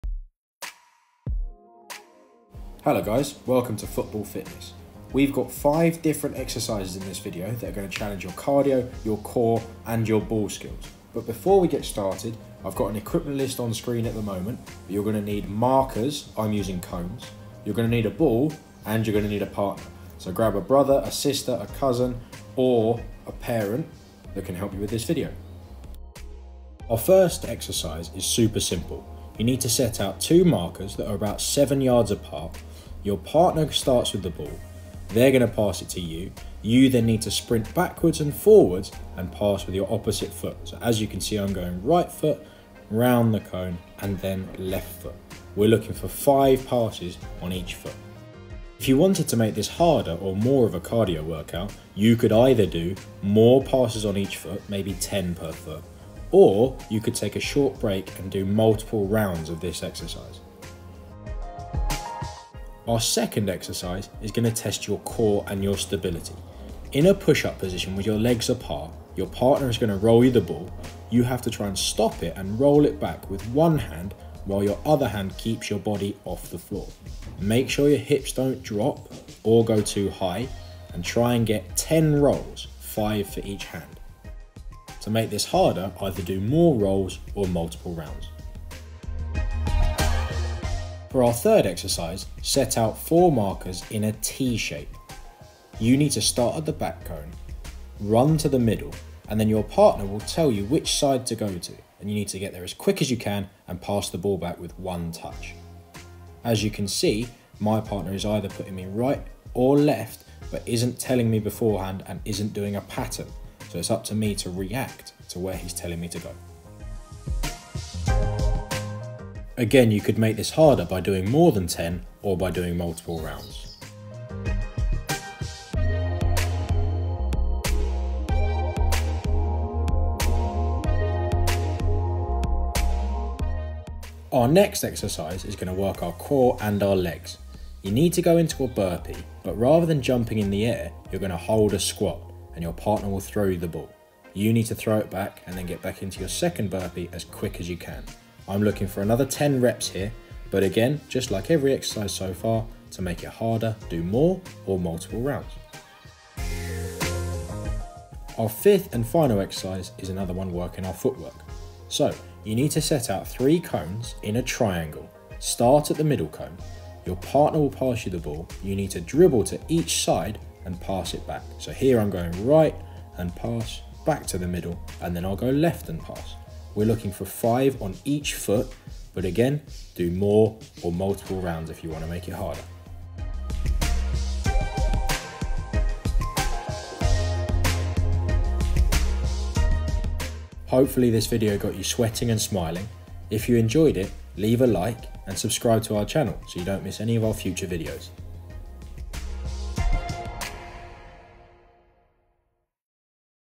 FOOTBALL FITNESS!! - simple exercises to do at home!
Grab a partner and a ball to keep active with these 5 simple fitness challenges that you can do from home!

Facebook: Josh Evans Soccer School